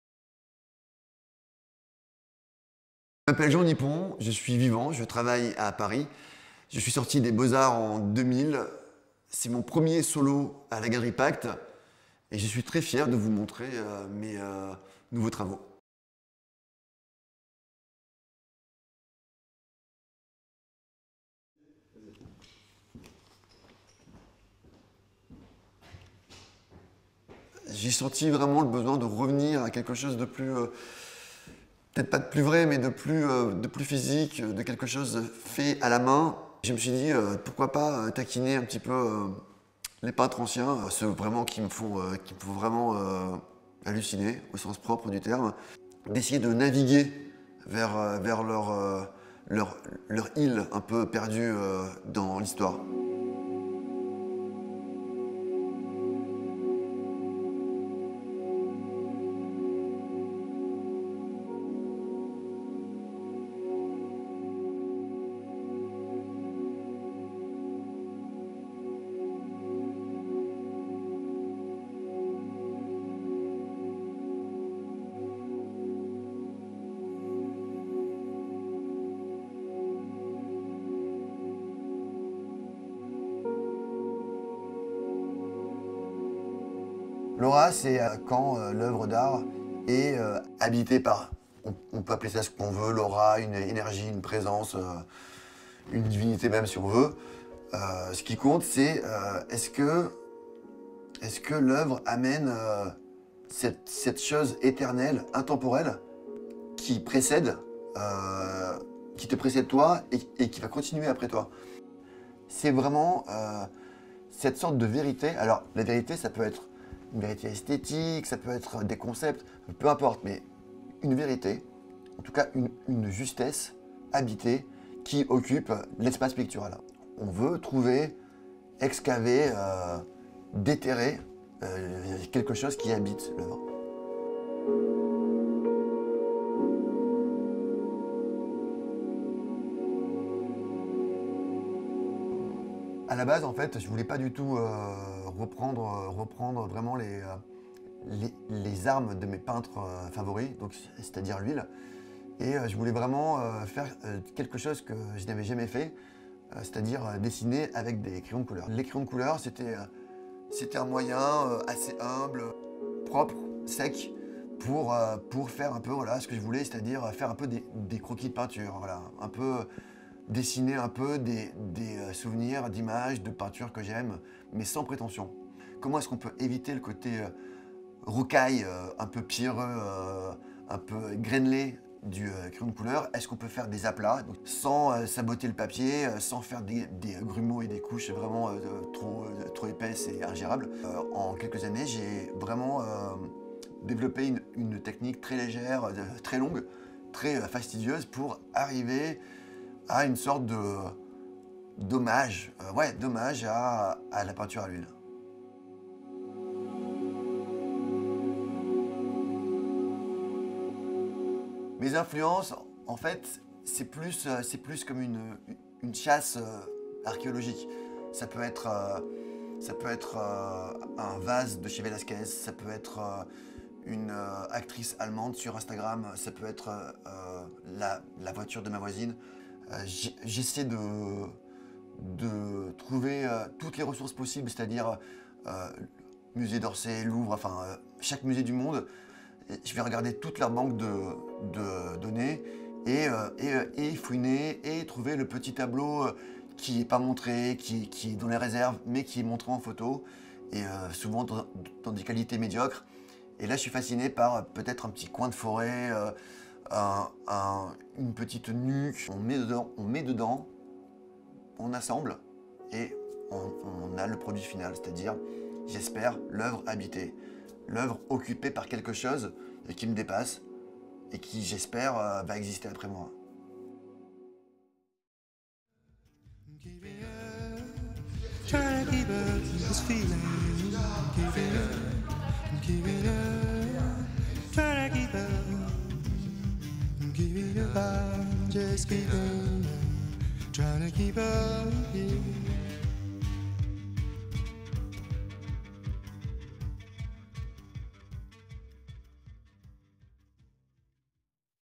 Who is Jean Nipon? Discover the artist behind the drawings
Jean Nipon (born in 1977, lives and works in Paris) is a French artist who graduated in 2000 from les Beaux-Arts in Bordeaux. After a career as a musician that made him famous on the international scene, he dedicated himself exclusively to drawing and had his first-ever solo exhibition at Galerie PACT.

Nipon has been influenced by masters of ancient painting such as Piero della Francesca, Nicolas Poussin, Georges De La Tour or Ingres, and portrays contemporary characters using rare colored pencils found around the world. In his drawings, one can recognise items from the past, such as Balthus’ room, a chest of drawers of Vermeer or a jar from Velázquez.